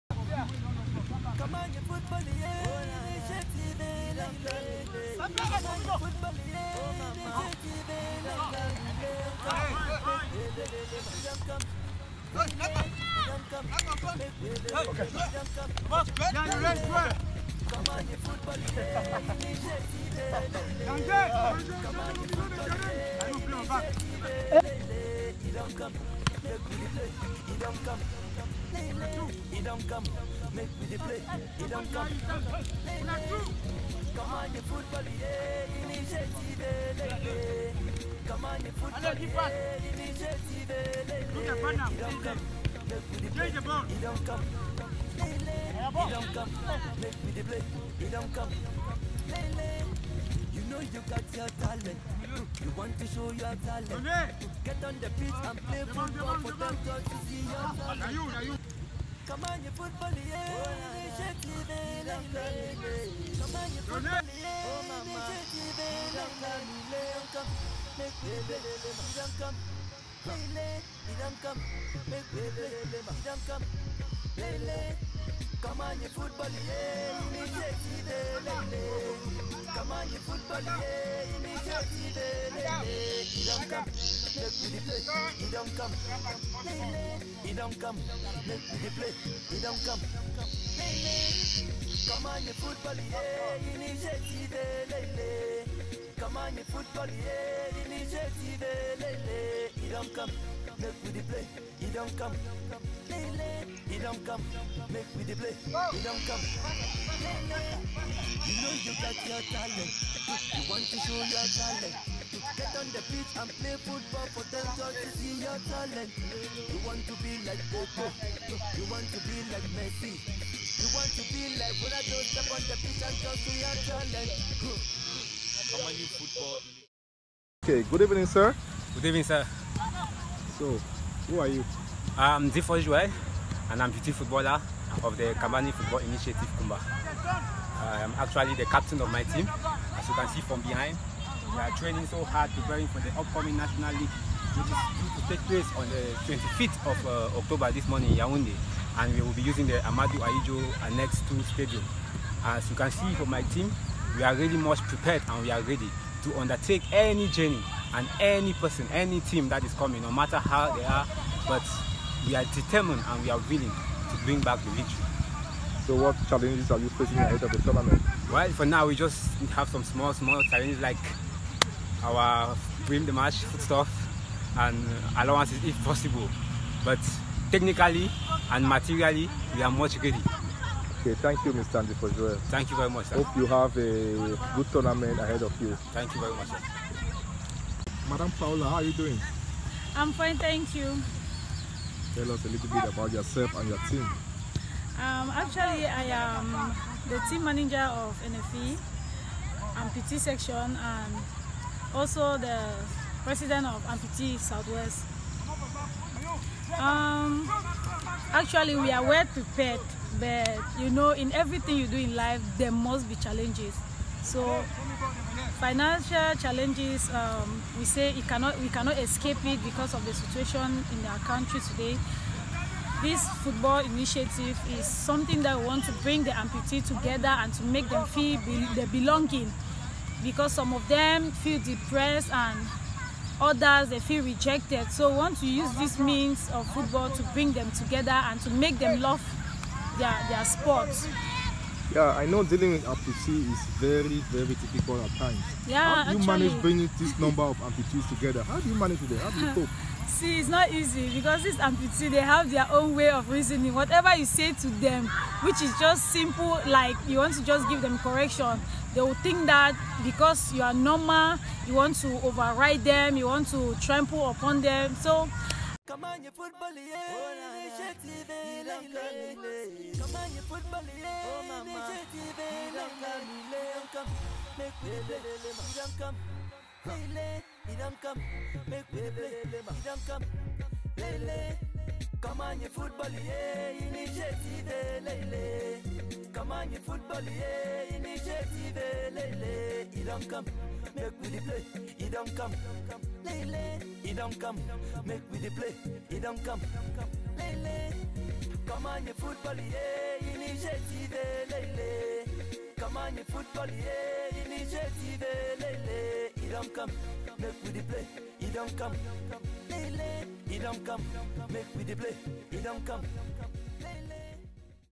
NFI Amputee Football Club in training
The Amputee team of the Nkamanyi Football Initiative, NFI AFC in training ahead of the first ever national Amputee Football League in Cameroon.